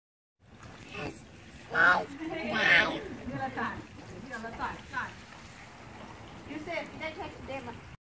It's slide time (water version)
On a fiery hot day, what does a hero do but cool off in some water. Bat-Sef takes a break from his cooling off to give us a message.